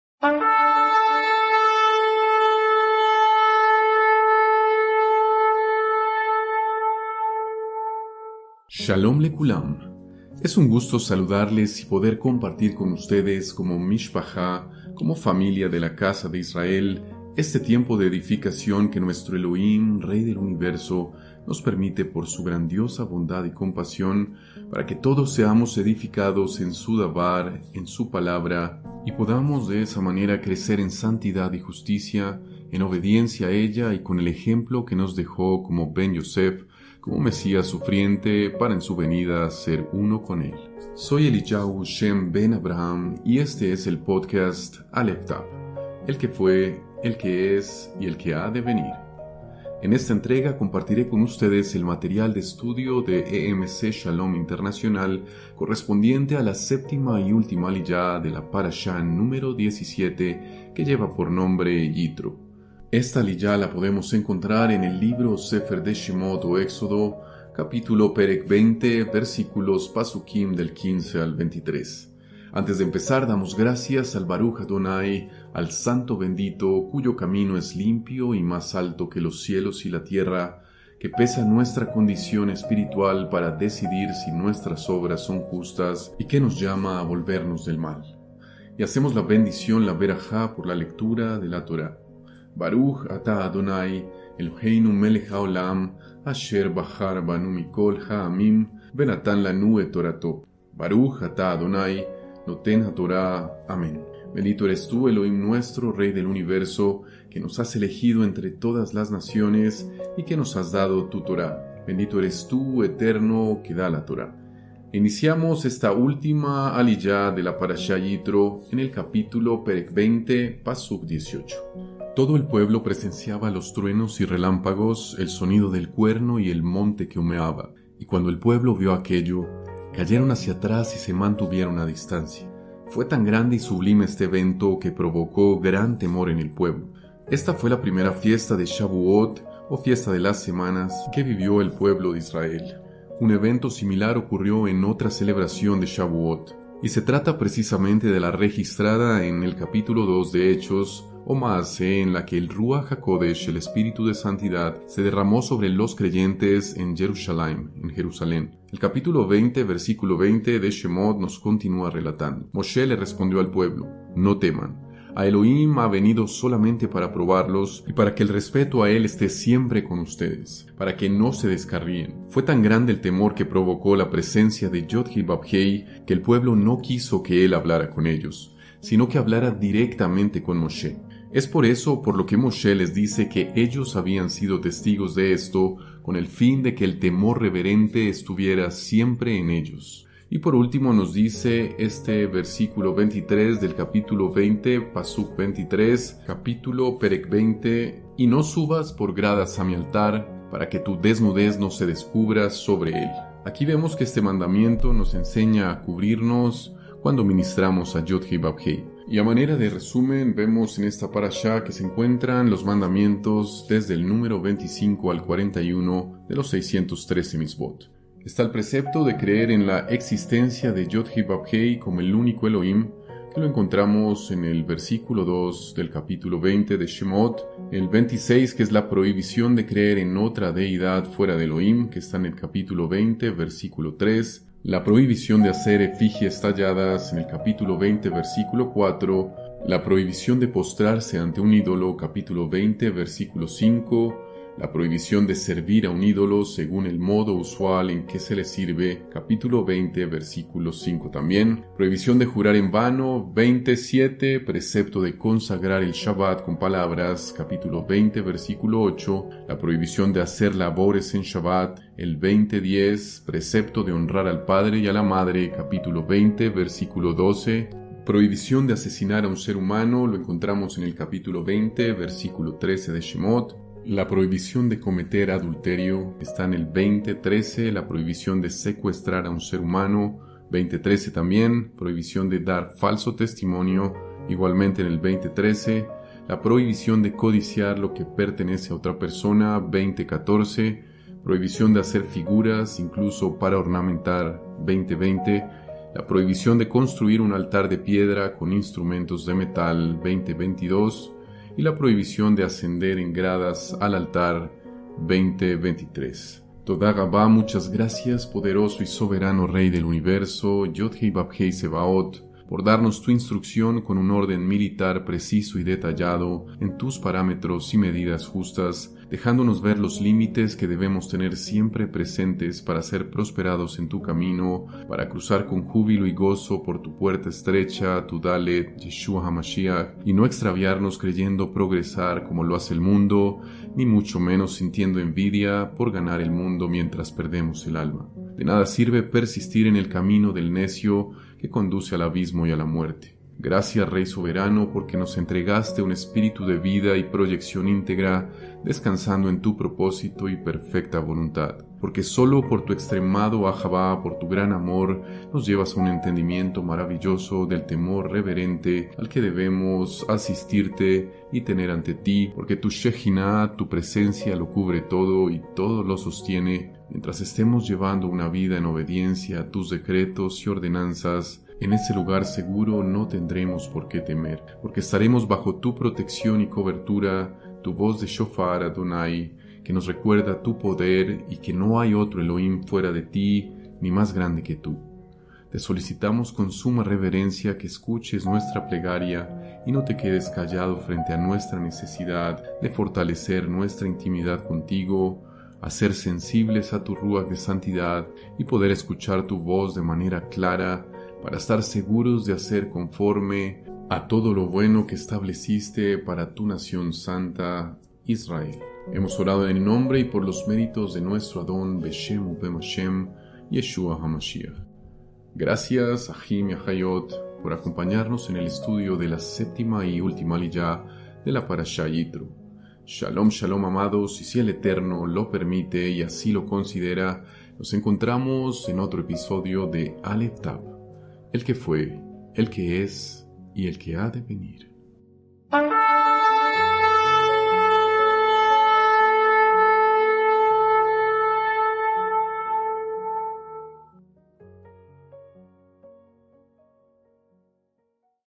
Alef Tav Séptima Aliyá Yitro
Narrado por: Eliyahu Shem Ben Avraham
PARASHÁ - PORCIÓN # 17 《YITRÓ…》*
7a Aliyá - (Ascenso).     ÉXODO 20:18-26.

18 (15) Todo el pueblo presenciaba los truenos y relámpagos, el sonido del cuerno y el monte que humeaba; y cuando el pueblo vio aquello, cayeron hacia atrás y se mantuvieron a distancia.

19 (16) Le dijeron a Mosheh: “Háblanos tú, que no nos hable Elohim, no sea que muramos”.

20 (17) Mosheh le respondió al pueblo: “No teman; ha’Elohim ha venido solamente para probarlos, y para que el respeto a él esté siempre con ustedes, para que no se descarríen”.

21 (18) De modo que el pueblo se mantuvo a la distancia, mientras Mosheh se acercó a la espesa nube donde estaba ha’Elohim.

22 (19) Y Yahweh le dijo a Mosheh: Así les dirás a los yisraelitas: Ustedes mismos vieron que les hablé desde el mismo cielo:

23 (20) Por lo tanto, a mi lado no harán deidades de plata, ni se harán ninguna deidad de oro.

24 (21) Háganme un altar de tierra y sacrifiquen sobre él sus ofrendas quemadas y sus ofrendas de bienestar, sus ovejas y sus bueyes; en todo lugar donde yo haga que se mencione mi nombre vendré a ti y te bendeciré.

25 (22) Y si me haces un altar de piedras, no lo construyas de piedras labradas; porque al emplear tu herramienta en él lo habrás profanado.

26 (23) No subas a mi altar por escaleras, para que no se exponga en él tu desnudez.